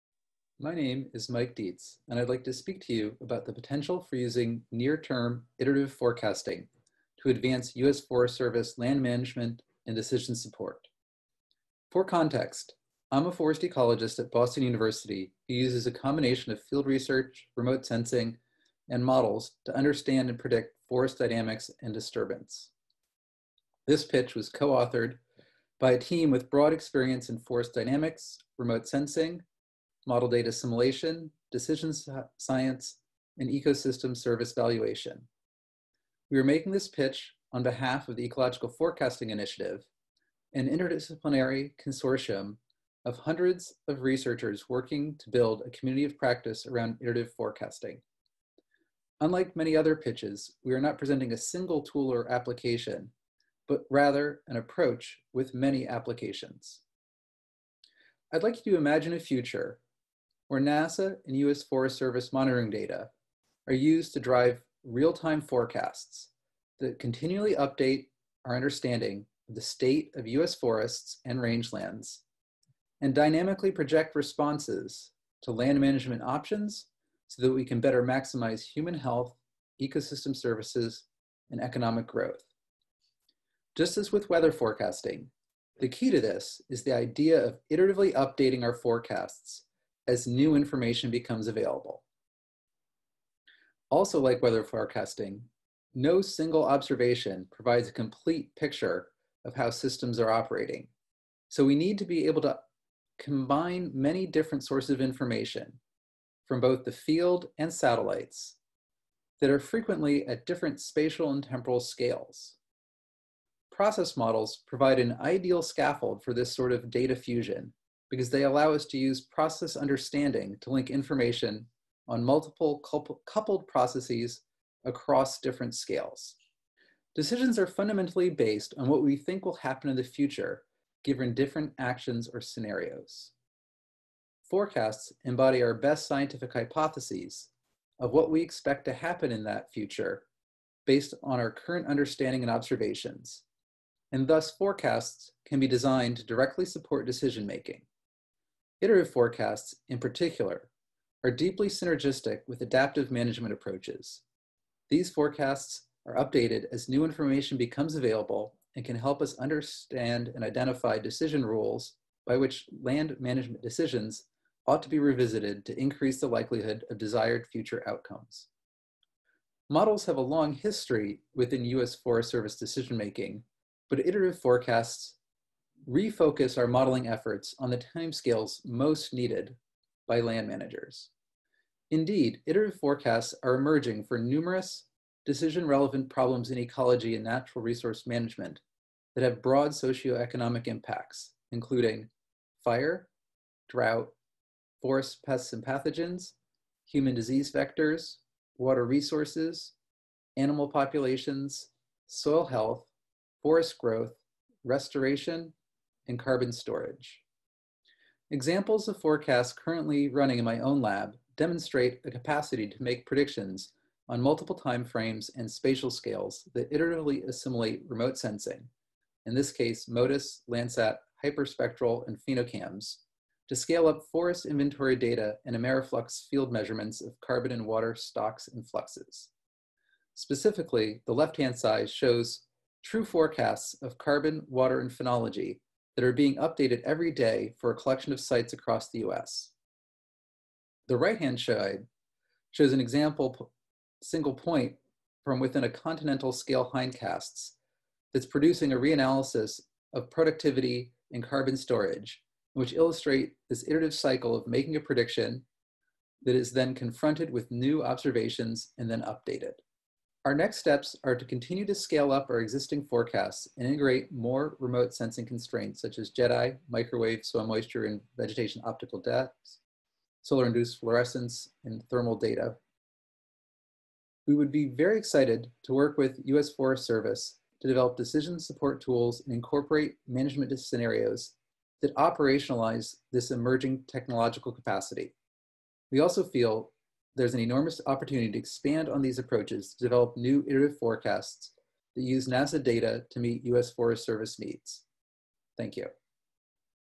Potential for near-term iterative forecasting to advance USFS land-management and decision support
Imagine a future where NASA and USFS monitoring data are used to drive real-time forecasts that continually update our understanding of the state of US forests and rangelands and dynamically project responses to land-management options so that we can better maximize human health, ecosystem services, and economic growth. Presentation made by Michael Dietze and the Ecological Forecasting Initiative for the USFS - NASA Virtual Pitchfest, June 3, 2020.